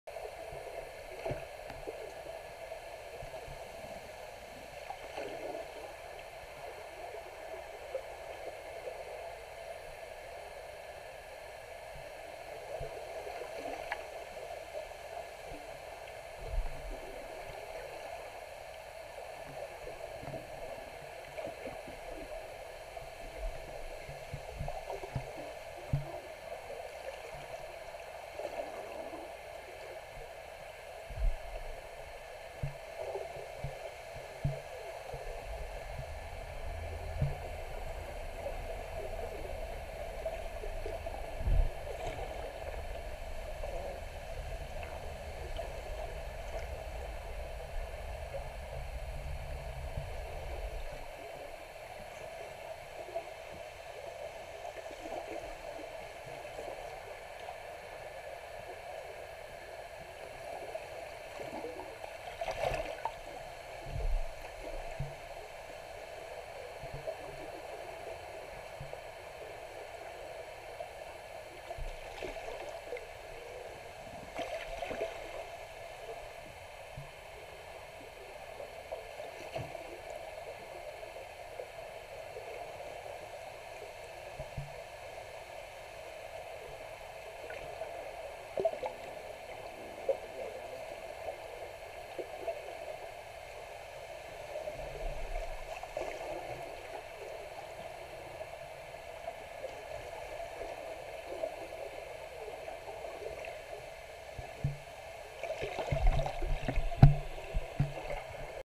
Soothing Salmos
Enjoy the Atlantic Salmon swim around, I really love the water sounds.